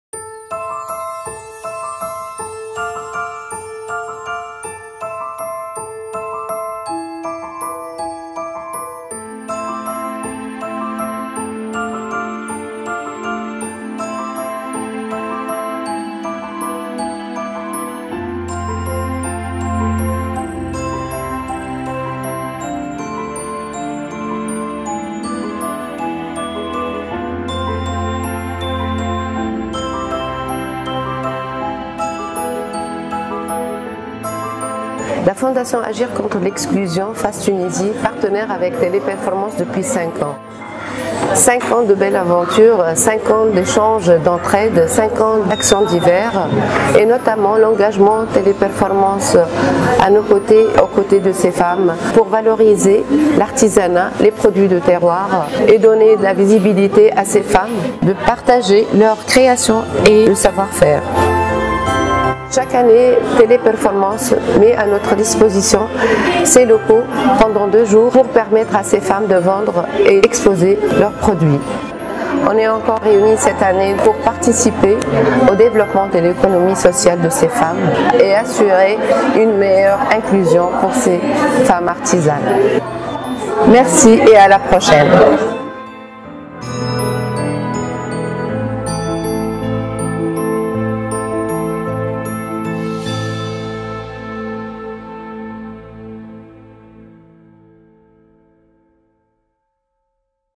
Rencontre avec Ahlem Chakroun chargée projets de l'association Face Tunisie
"Rencontre avec Ahlem Chakroun chargée projets de l'association Face Tunisie à l'occasion de notre 5ème action de soutien dans le cadre de ""La Tournée des produits de terroir Face aux entreprises""
En effet, TP a accueilli les 7 et 9 décembre sur nos centres de Kheireddine Pacha et Ben Arous plus de 20 exposantes pour présenter une panoplie de produits agricoles, produits de terroir et créations artisanales avec de belles touches de modernité
Cette action pérenne a pour objectif de renforcer notre positionnement responsable en Tunisie et traduire l'implication de notre entreprise dans le soutien des femmes artisanes pour contribuer à leur autonomisation
tout en permettant de promouvoir le métier d'artisan et encourager la consommation de produits tunisiens